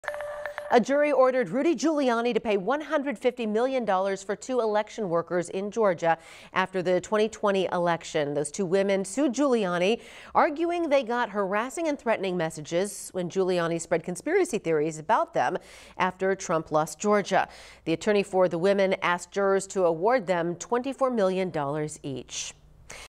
Giuliani ordered to pay $148M in damages for defaming Georgia election workers
A federal judge ruled Giuliani had defamed Ruby Freeman and Shaye Moss when he falsely claimed they had engaged in election fraud in 2020.